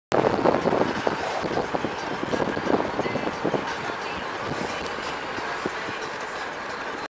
Riding By Author William Kennedy’s House
Riding in my father’s car and going past author William Kennedy’s house. William Kennedy’s the man who wrote the book “Ironweed” which later became a movie that starred Jack Nicholson and Meryl Streep. It was also comedian Nathan Lane’s first movie, though he had a serious role in the film.